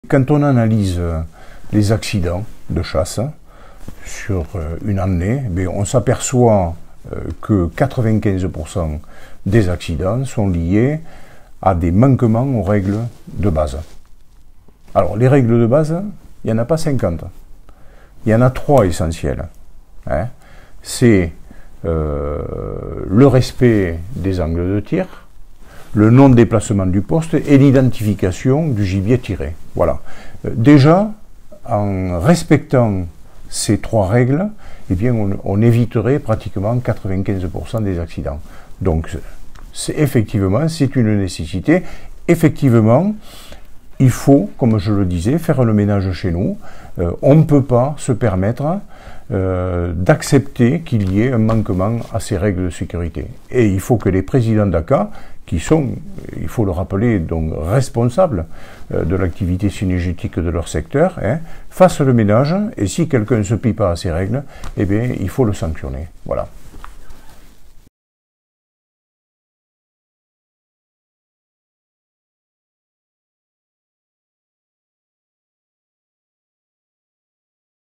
Jean-Pierre SANSON, à l'occasion de la #100 de la FDC66, alerte sur la sécurité à la chasse
Jean-Pierre SANSON, Président FDC66,  rappelle les règles élémentaires de sécurité à la chasse.
Il en fait depuis plusieurs années son cheval de bataille et appelle à aucune indulgence devant tout manquement.